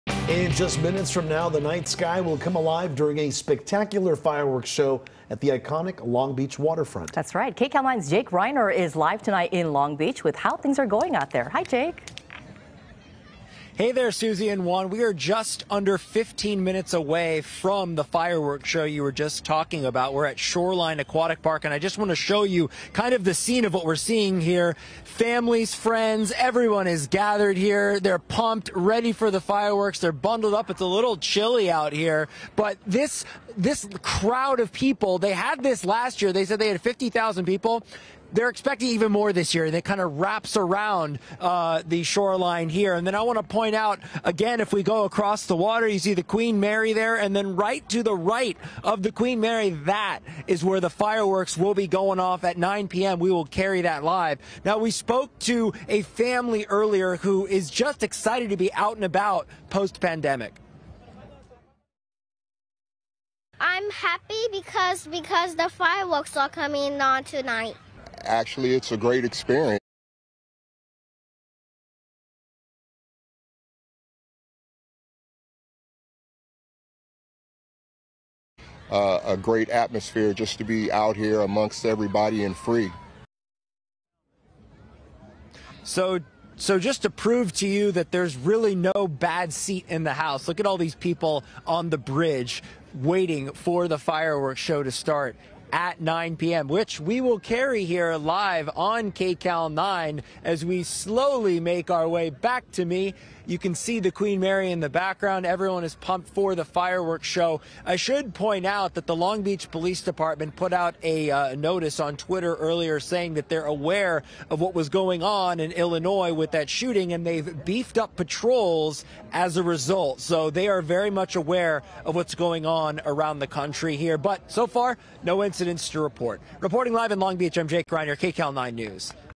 Many wait for the fireworks to light up the Long Beach waterfront
Many await the spectacular fireworks show that features the iconic Queen Mary.